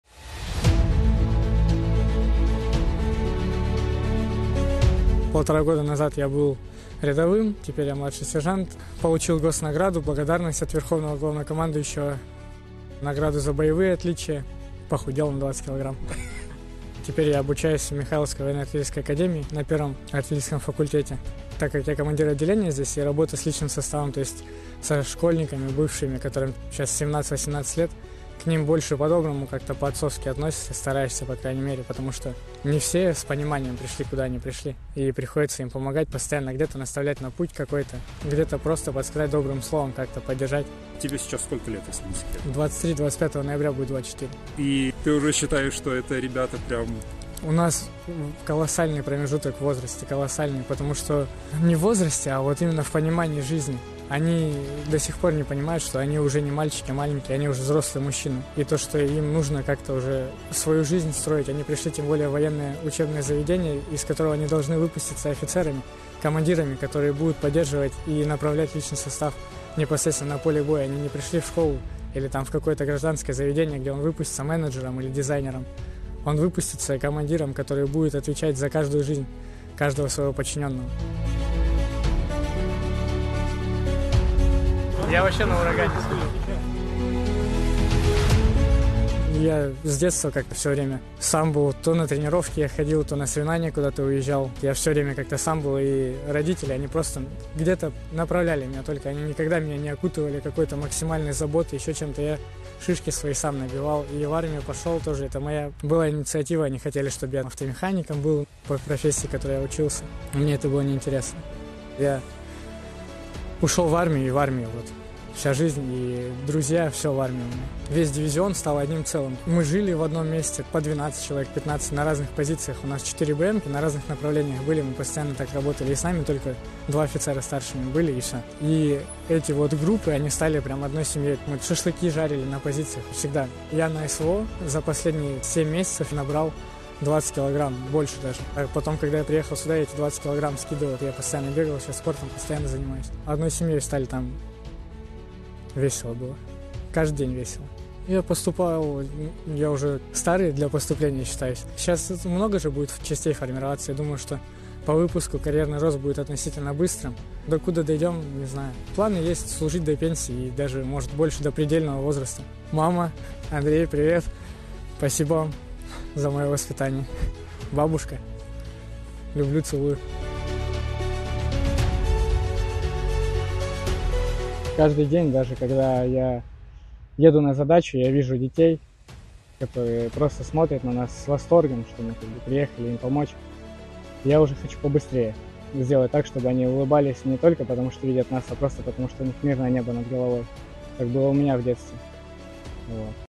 •🇷🇺С Днём ракетных войск и артиллерии✊/19ноября
🇷🇺Младший сержант Александр Олейник — летом прошлого года в зоне спецоперации,  служил в расчете «Урагана». Сейчас он — курсант Михайловской военной артиллерийской академии.

Морозным ноябрьским днём на полигоне в Луге Александр рассказывает о том, что за это время поменялось в его жизни. Теперь он,как командир отделения,  по-отечески воспитывает младших товарищей. Ведь в свои 23 года он уже командир с боевым опытом.

Поздравляю Александра и всех причастных с Днём ракетных войск и артиллерии! ✊🏻